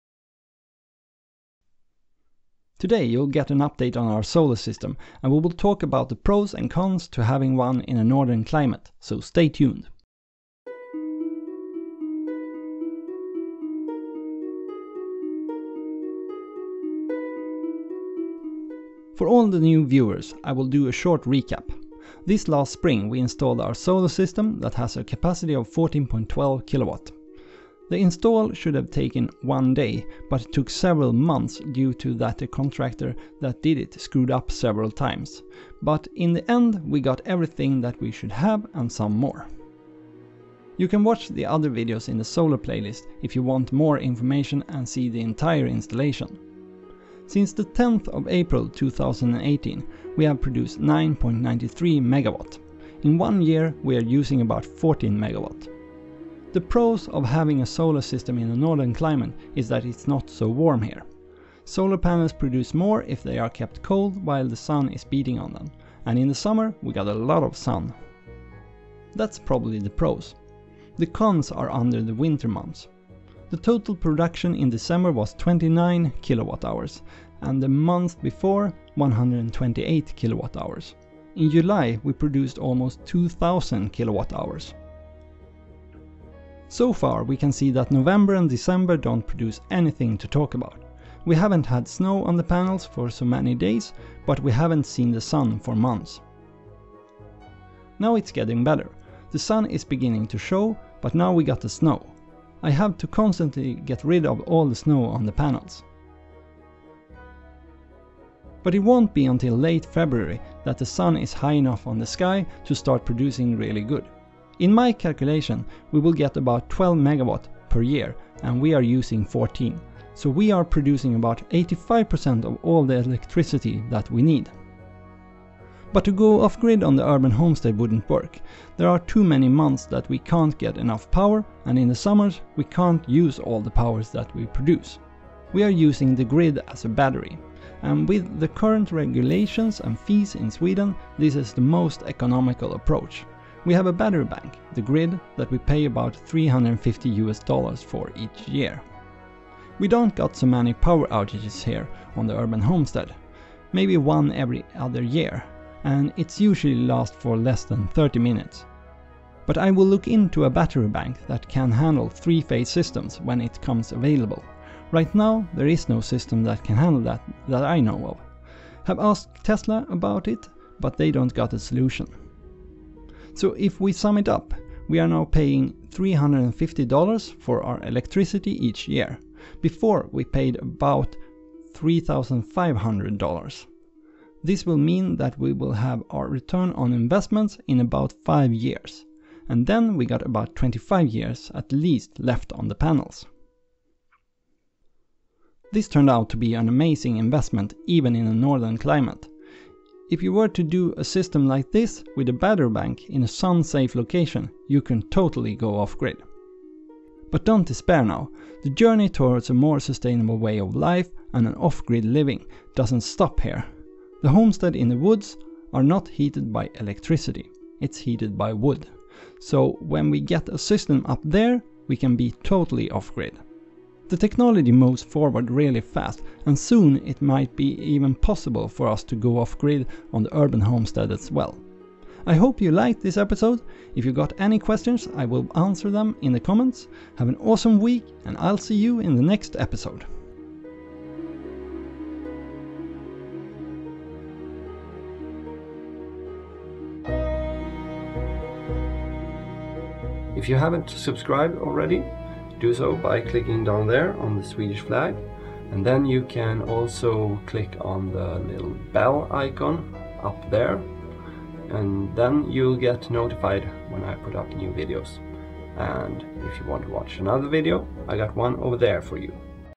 Solar Power In Northern Climate
Does solar panels work in northern climate? Today we will look in to Solar Power In Northern Climate. We have had our solar system for almost a year now and its working great. Solar power is the future even in an northern climate. Our solar panel production in one year is calculated to 12000 kWh and we are using 14000 kWh. We are producing 85% of our electricity.

This is a step to off grid living but that will be on the homestead in the woods. Here on the urban homestead it will take time before we can do it but in the mean time we are making a smaller carbon footprint on our enviroment and that feels great.

I would love to do a full solar power documentary in the future with all the details in each step. Meanwhile you can look at the solar playlist for the whole instalation.

My Equipment:
4K camcorder
Wireless microphone (ON SALE)
Editing software